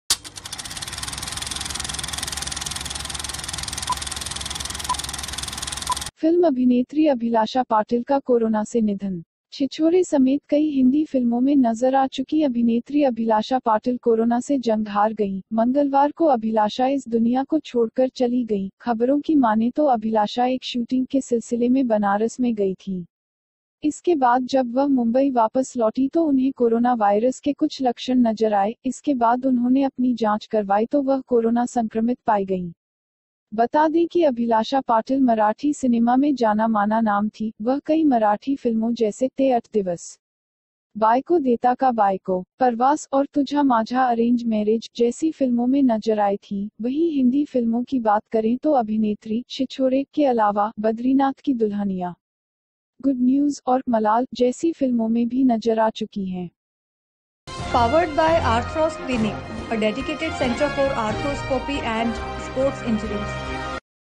Film Actress Abhilasha Patil का कोरोना से निधन, सुशांत सिंह राजपूत की फिल्म 'छिछोरे' में किया था काम
Film actress Abhilasha Patil died of Corona, worked in Sushant Singh Rajput's film 'Chichhore'

Actress Abhilasha Patil, who has appeared in many Hindi films including Chhichhore, lost the battle to Corona. Abhilasha left this world on Tuesday. According to the news, Abhilasha went to Benaras in connection with a shooting.

After this, when she returned to Mumbai, she noticed some symptoms of corona virus. After this, when she conducted her investigation, she was found to be corona infected.

Let me tell you that Abhilasha Patil was a well-known name in Marathi cinema. She appeared in many Marathi films such as 'Te Atha Divas', 'Byco Dita Ka Byco', 'Parwas' and 'Tujha Majha Orange Marriage'. Talking about Hindi films, in addition to actress 'Chhichhore', she has also appeared in films like 'Badrinath Ki Dulhania', 'Good News' and 'Malal'.
फिल्म अभिनेत्री अभिलाषा पाटिल का कोरोना से निधन, सुशांत सिंह राजपूत की फिल्म 'छिछोरे' में काम किया था

छिछोरे' समेत कई हिंदी फिल्मों में नजर आ चुकीं अभिनेत्री अभिलाषा पाटिल कोरोना से जंग हार गईं। मंगलवार को अभिलाषा इस दुनिया को छोड़कर चली गईं। खबरों की मानें तो अभिलाषा एक शूटिंग के सिलसिले में बनारस में गई थीं।

इसके बाद जब वह मुंबई वापस लौटीं तो उन्हें कोरोना वायरस के कुछ लक्षण नजर आए। इसके बाद उन्होंने अपनी जांच करवाई तो वह कोरोना संक्रमित पाई गईं।

बता दें कि अभिलाषा पाटिल मराठी सिनेमा में जाना-माना नाम थी। वह कई मराठी फिल्मों जैसे 'ते अठ दिवस', 'बायको देता का बायको', 'परवास' और 'तुझा माझा अरेंज मैरेज' जैसी फिल्मों में नजर आई थीं। वहीं हिंदी फिल्मों की बात करें तो अभिनेत्री 'छिछोरे' के अलावा 'बद्रीनाथ की दुल्हनिया', 'गुड न्यूज' और 'मलाल' जैसी फिल्मों में भी नजर आ चुकी हैं।